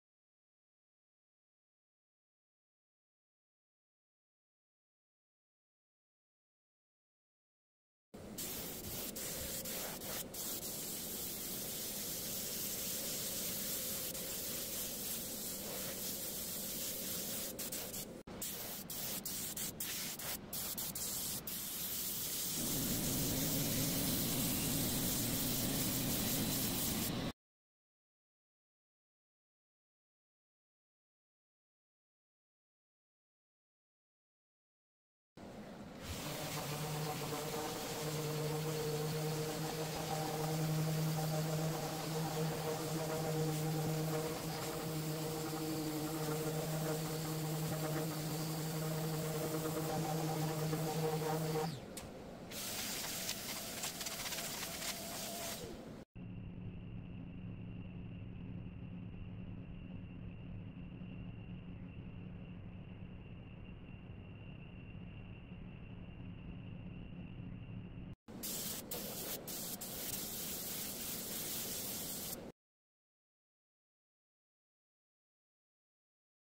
Facts. Tyson Photo Carve
another one for the personal collection.   Tyson is a beast.

52° line angle
115% spacing
.07 depth
.09 step over
60° V Bit
1/2" mdf unsealed

order of operation after the carve:
seal with shellac
paint with dark colored latex paint.
let dry
Sand with 120 grit sandpaper.
optional seal with clear poly  or what ever you have.